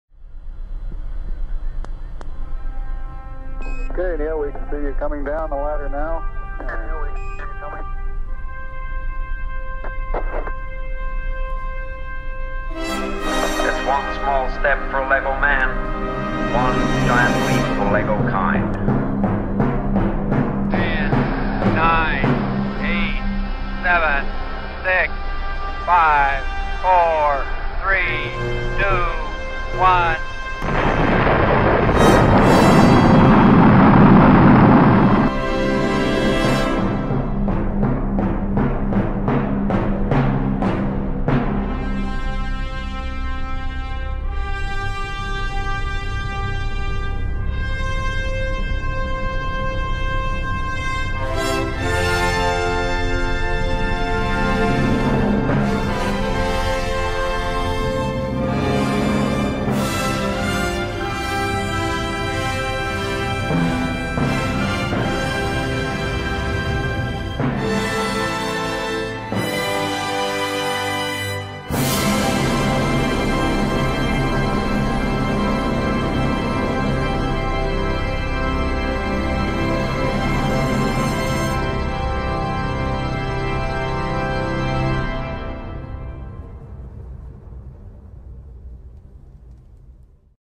Jenny Hammond Space Agency - Launching June 2019
In June 2019, 4 brave Lego astronauts will boldly go where no lego man has gone before...